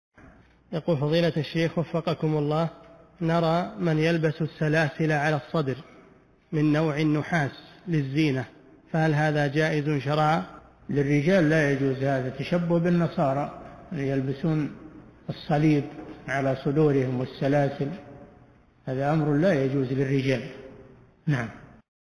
[80 -2802] ما حكم لبس الرجل لسلسلة من النحاس على الصدر من أجل الزينة؟ - الشيخ صالح الفوزان
"السؤال: نرى من يلبس السلاسل على الصدر من نوع النحاس للزينة، فهل هذا جائزٌ شرعًا؟
الجَوَابُ: للرجال لا يجوز هذا تشبُّه بالنصارى يلبسون الصليب على صدورهم والسلاسل، هذا أمرٌ لا يجوز للرجال.

للاشتراك في قناة الشيخ صالح الفوزان
سؤال وجواب من شرح أخصر المختصرات ( 2802 ) مقطع

للاستفسار والإقتراحات والدعم
رئيس الفريق
لمشروع كبار العلماء